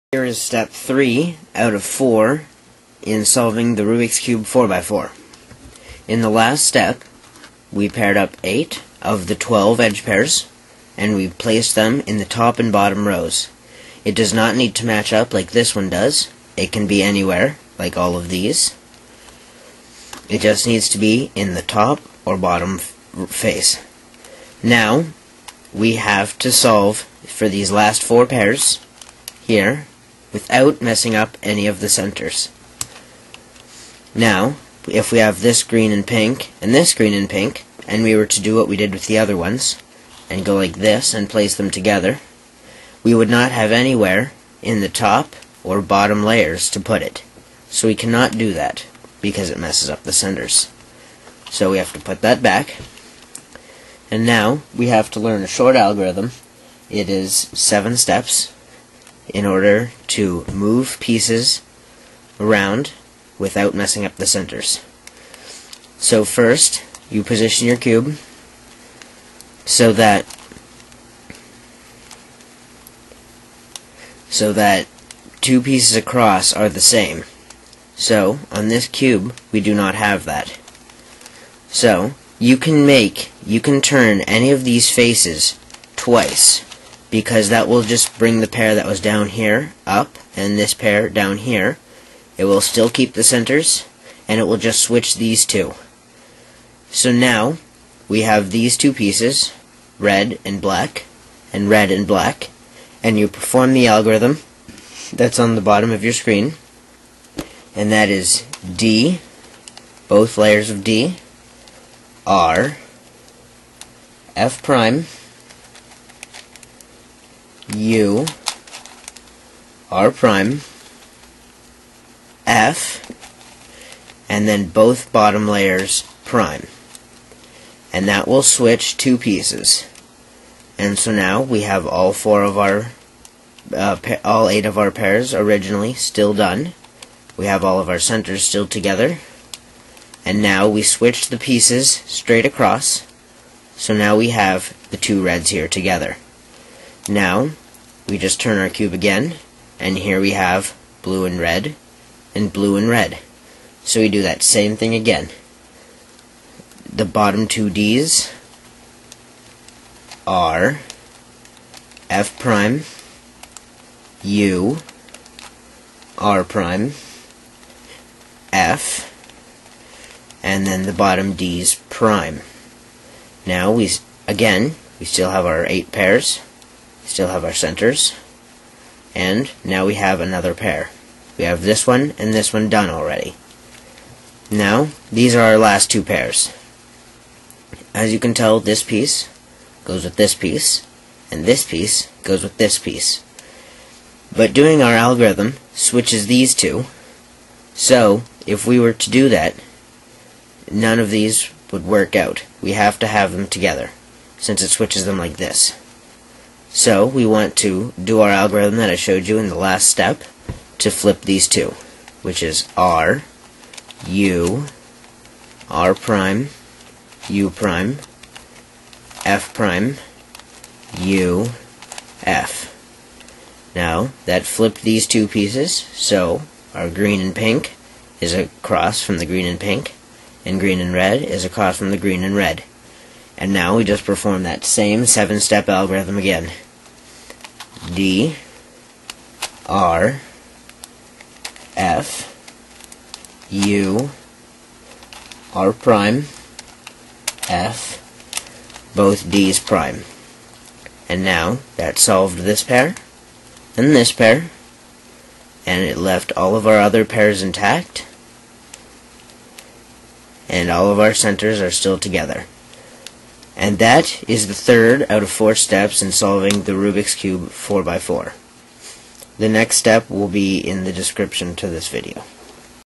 How to Solve a Rubik's Cube 4x4 Step 3
This is how to solve a Rubik's Cube 4x4 step 3. Which is pairing the last 4 edge pairs.